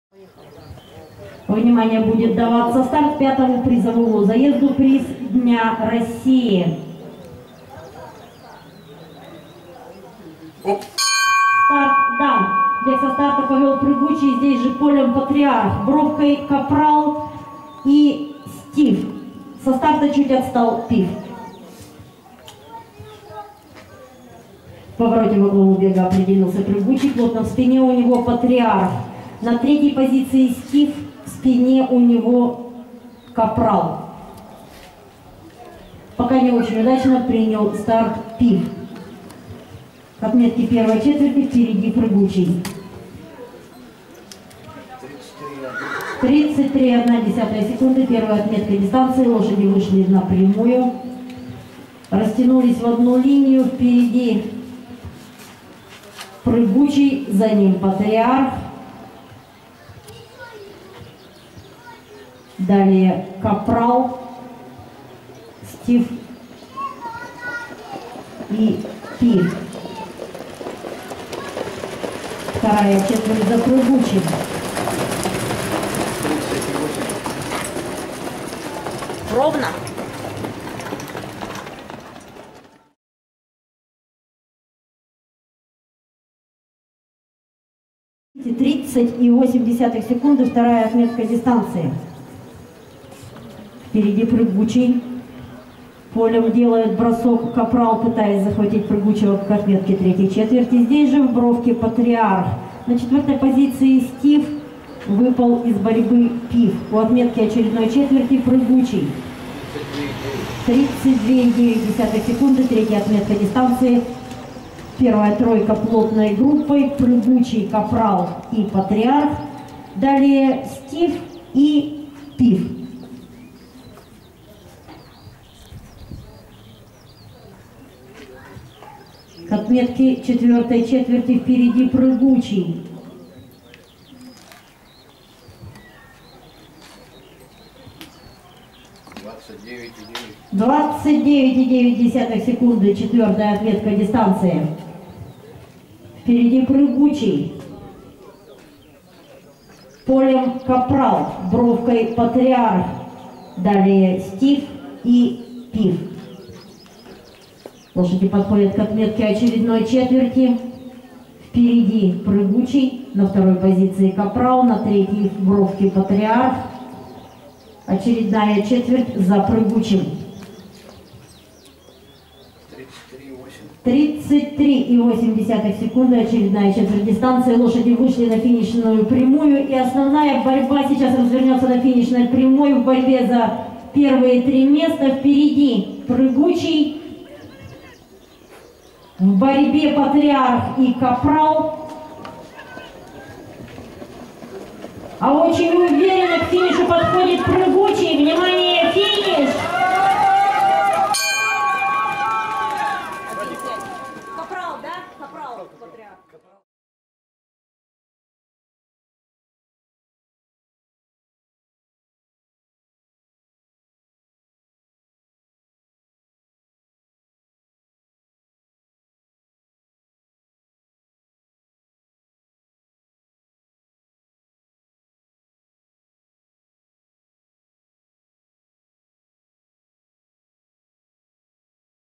Заезд 5. Приз Дня России (традиционный)
Приз Дня России (традиционный) для лошадей старшего возраста рысистых пород, 2400 м
Висим 06.06.20